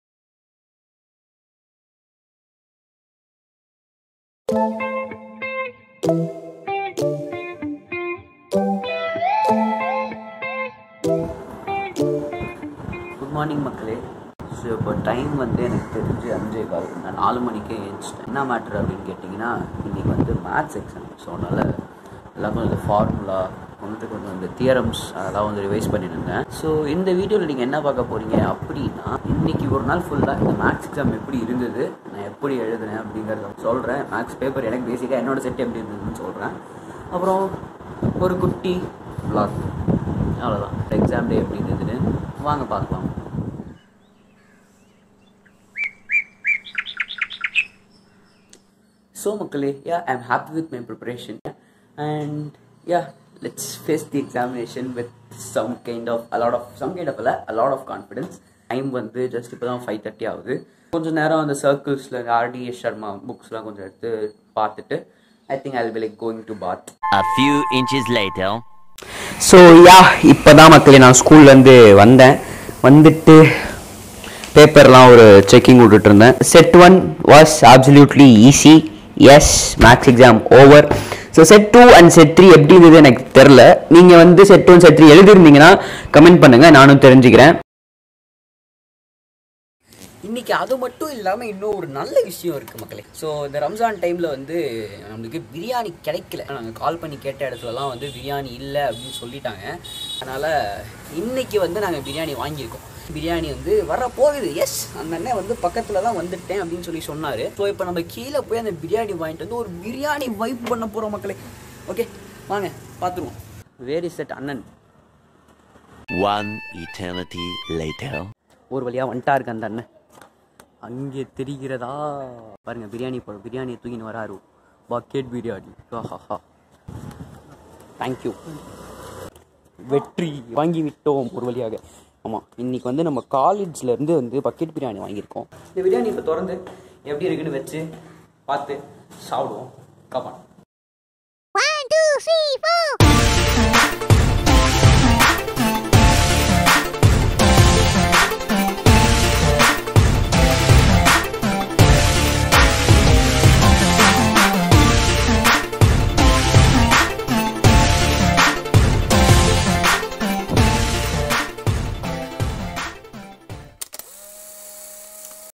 Exam day vlog & Vibing with biriyani 😋🤪 Tamil
indha short vlog la, yennoda exam day yepudi pochu nu therinjikonga makkale.

biriyani details :

mutton bucket biriyani (serves 4 to 5 people) : rs1600
order on khalids biriyani website!